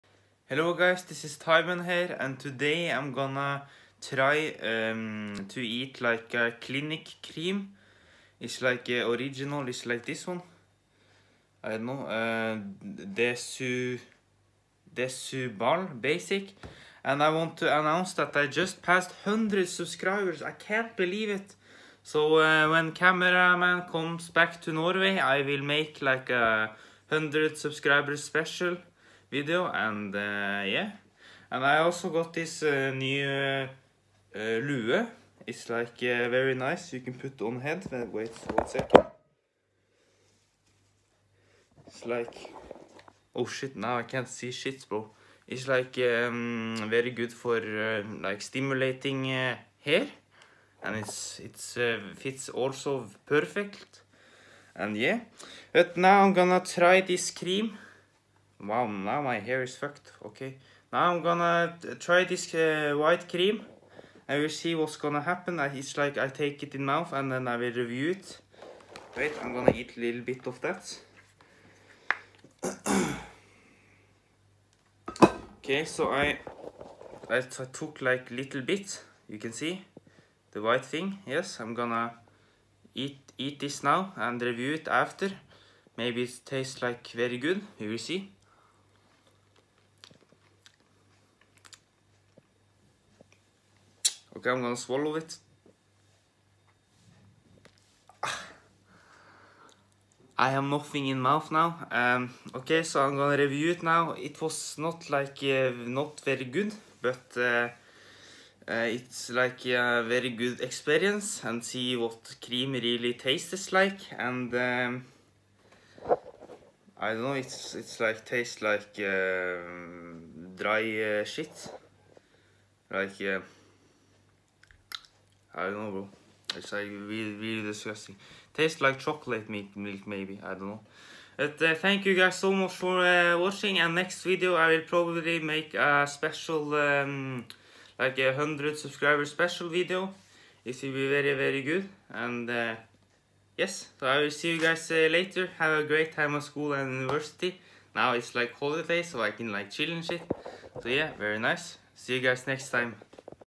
Tiedemann trying cream (spoiler alert: sjikolade milk
Remember to always have fun in life and make sure to not give a fuck of what people think about you 🙂 Pray to the Gods in valhalla and have fun hehe.